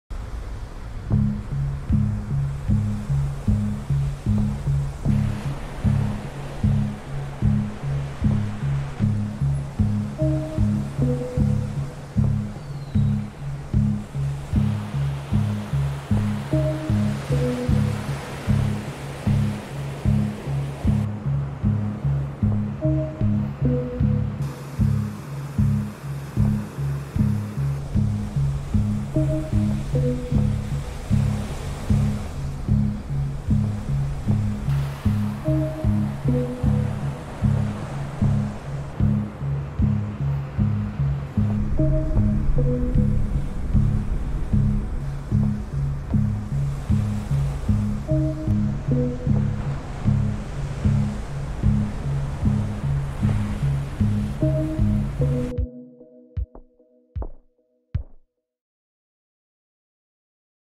Public digital artwork "wave" in Seoul, Korea by d'strict
"wave" with anamorphic illusion is a public digital media art in Gangnam-gu, Seoul, South Korea created by d'strict for COEX K-POP Square.
Video courtesy of d'strict